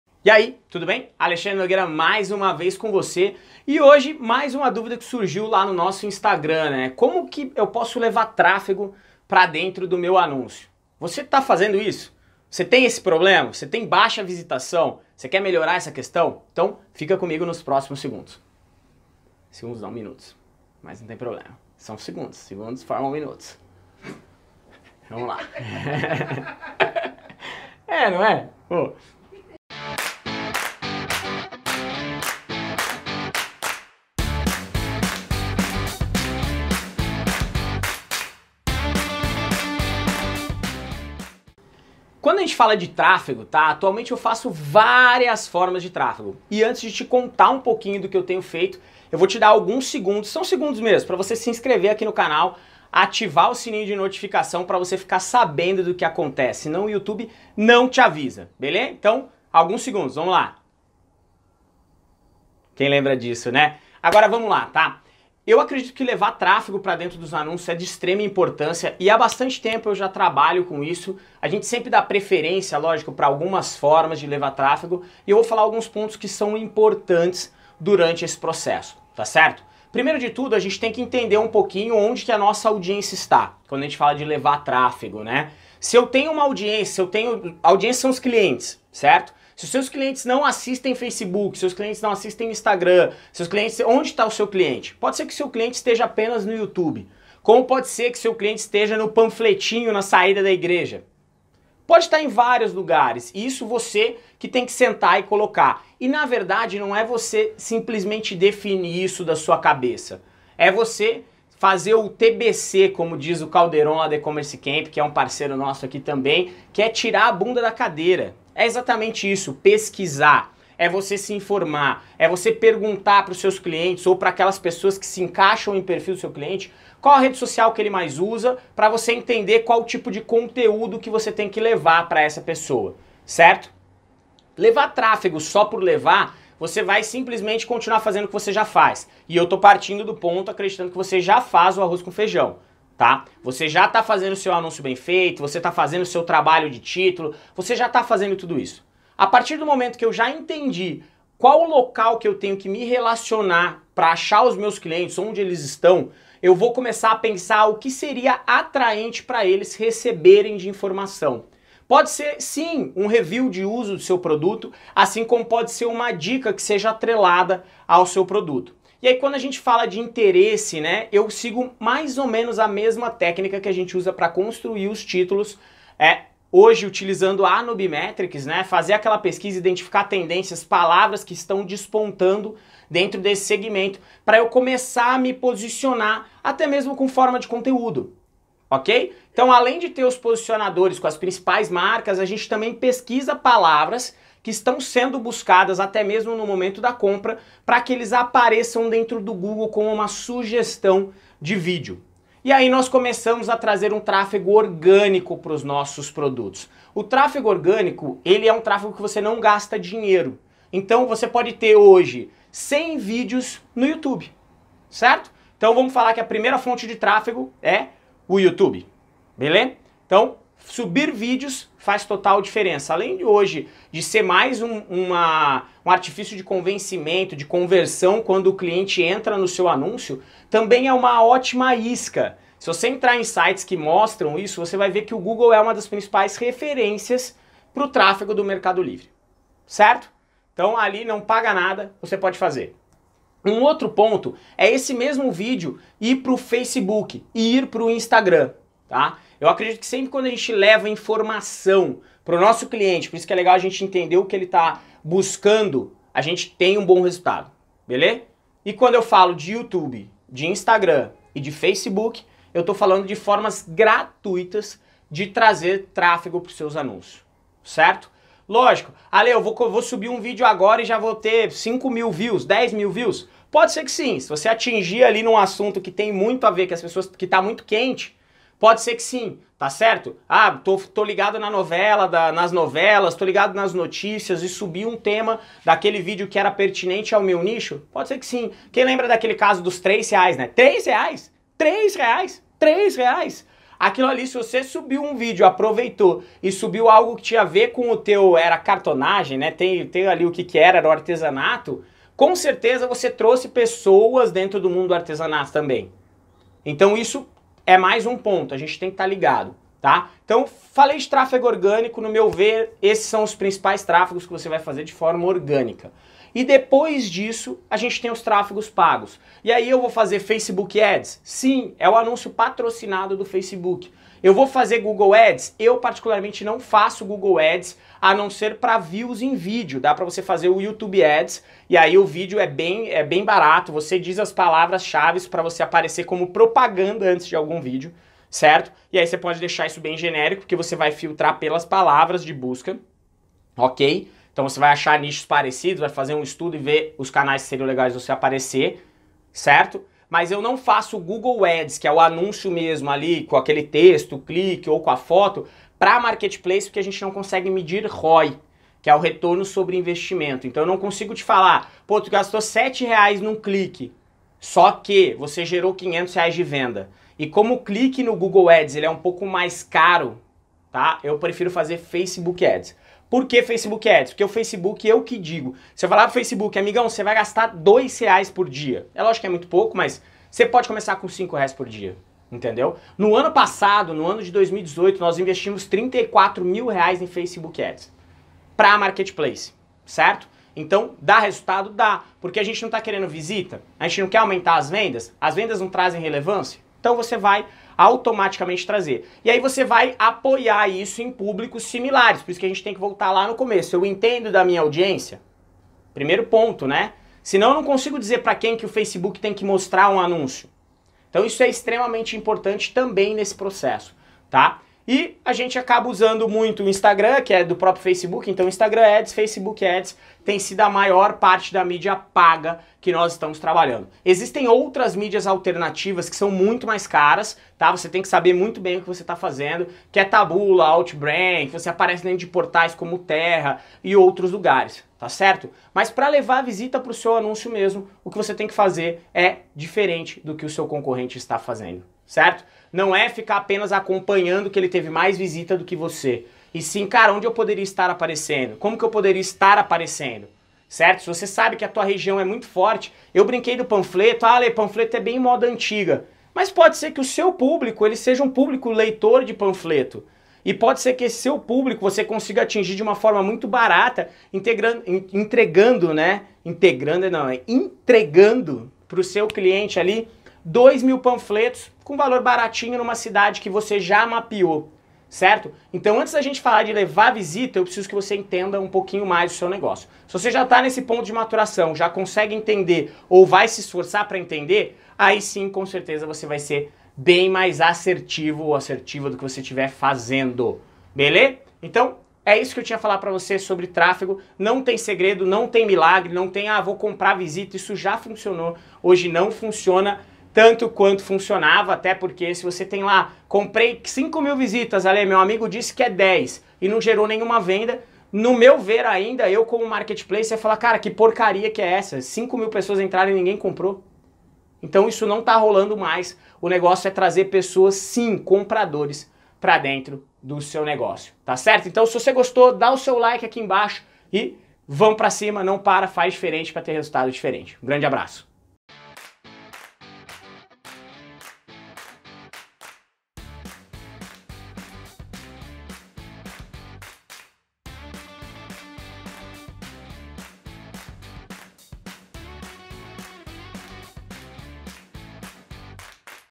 Levar Visitas no Mercado Livre e Marketplaces Facebook Ads Youtube Como Vender Mais Online
Como trazer tráfego para o seu anúncio.
já faz algum tempo que eu não espero o marketplace trazer as vendas para mim, nós temos um postura pró-ativa de prospecção de clientes.

Trabalhamos com tráfegos pagos e gratuitos que são direcionados para as ofertas que queremos posicionar alterando o foco quando atingimos o objetivo.

Trazer clientes para dentro dos seus anúncios, faz com que você tenha venda e as vendas trazem relevância.

Ao gerar tráfego para os marketplaces você também acelera o processo pois eles trabalharão o remarketing, buscando clientes que já visitaram seus anúncios.